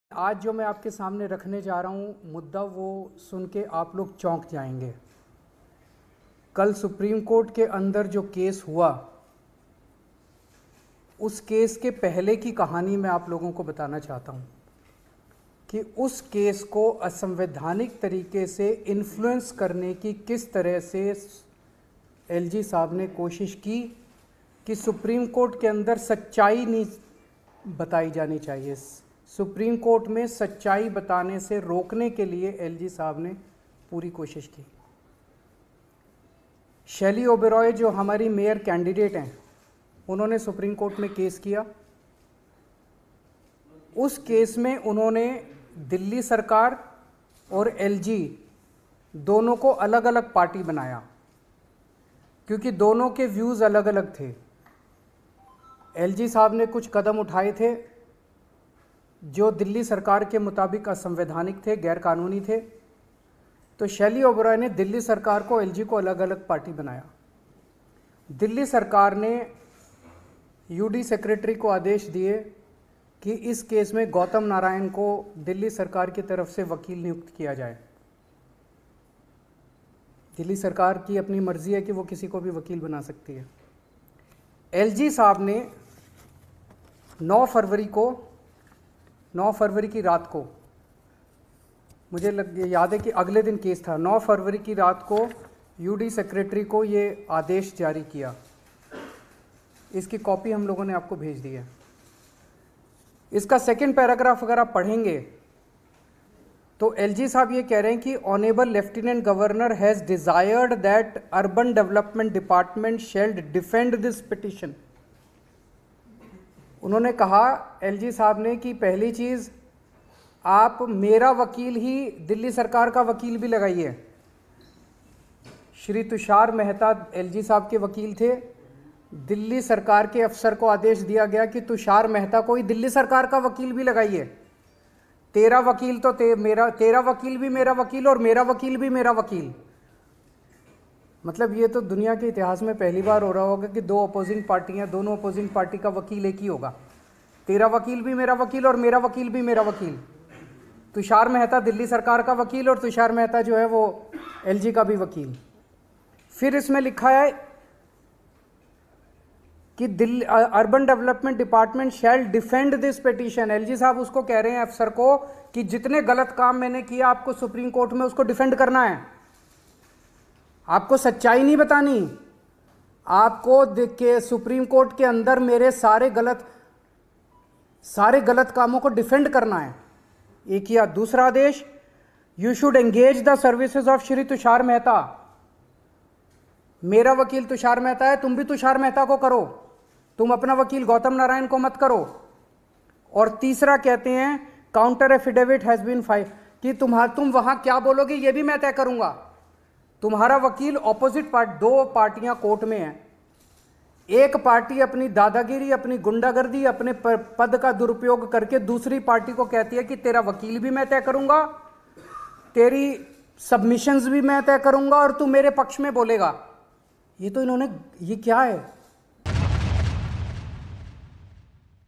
Delhi Politics: CM केजरीवाल ने LG पर लगाया शक्तियों के दुरुपयोग का आरोप | Arvind Kejriwal | AajTak
दिल्ली में संवैधानिक पदों पर आसीन दो शख्स सीएम केजरीवाल और एलजी वीके सक्सेना के बीच खींचतान खत्म होने का नाम नहीं ले रही है. दोनों को अक्सर हर मुद्दे पर एक दूसरे पर आरोप लगाते देखा जा सकता है. अब दिल्ली के सीएम केजरीवाल ने प्रेस कॉन्फ्रेंस कर एलजी पर सुप्रीम कोर्ट के आदेश की अवहेलना करने का आरोप लगाया है.

The tussle between CM Kejriwal and LG VK Saxena, two persons holding constitutional posts in Delhi, is not taking the name of ending. Both can often be seen accusing each other on every issue. Now CM Kejriwal of Delhi has accused the LG of disobeying the order of the Supreme Court by holding a press conference.